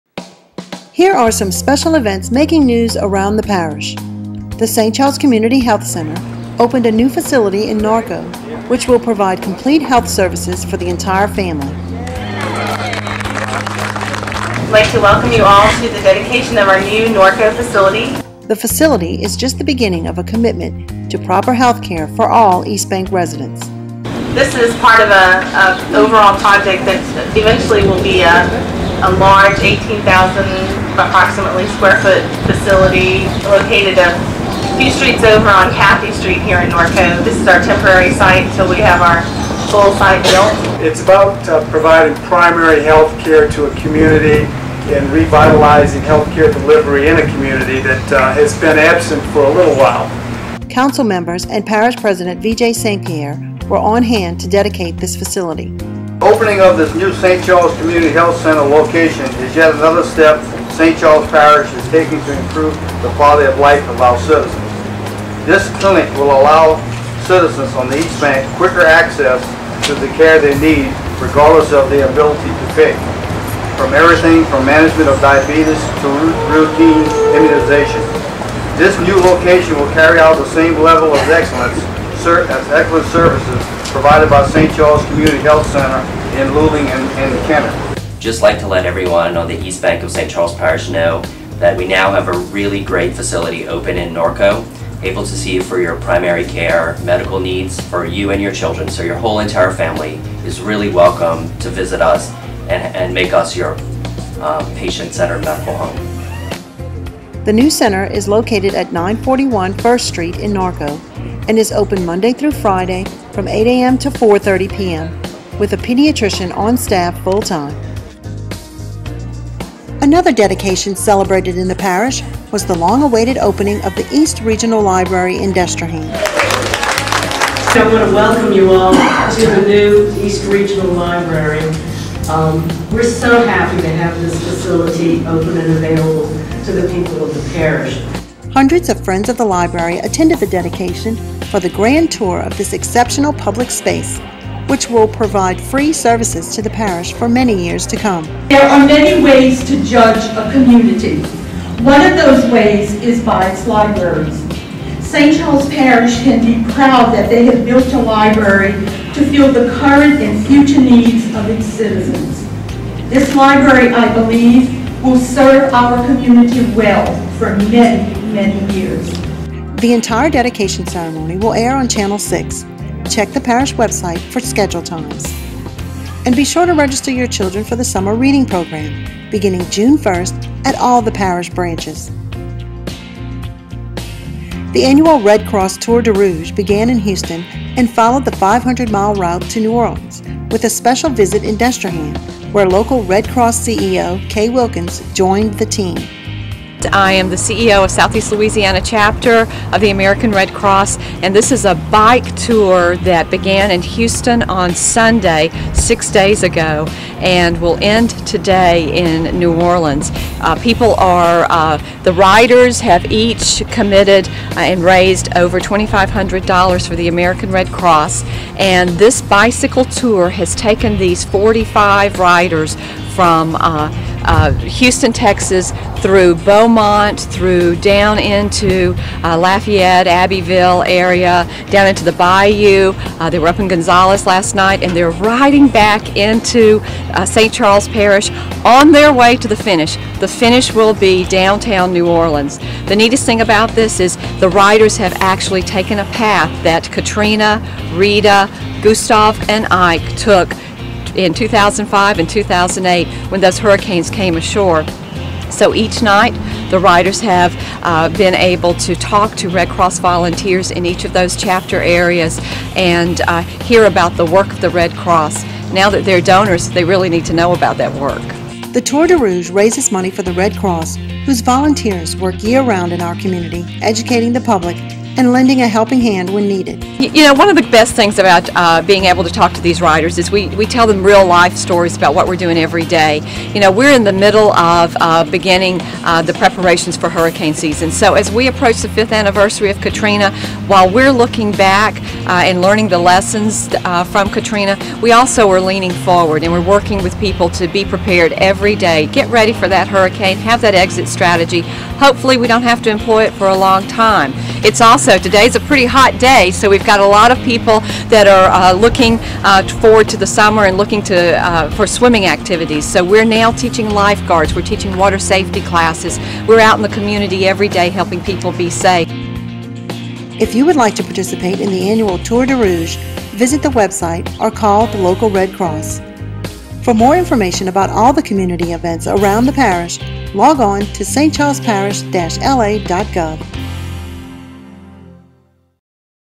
Around the Parish for May 14, 2010
Find out what happened in St. Charles Parish for the week of May 14, 2010... The St. Charles Parish Community Health Center opened a new facility in Norco, which will provide complete health services for the entire family. The facility is the continuation of a commitment to provide proper heath care for all East Bank residents. Council members and Parish President V.J. St. Pierre were on hand to dedicate this new facility... Another dedication celebrated in the parish was the long-awaited opening of the East Regional Library in Destrehan. Hundreds of friends of the library attended the dedication for a grand tour of this exceptional public space, which will provide services to the parish for many years to come... The annual Red Cross Tour de Rouge began in Houston and followed the 500 mile route to New Orleans, with a special visit in Destrehan, where local Red Cross COE Kay Wilkins joined the team. The Tour de Rouge raises money for the Red Cross, whose volunteers work year round in our community educating the public and lending a helping hand when needed...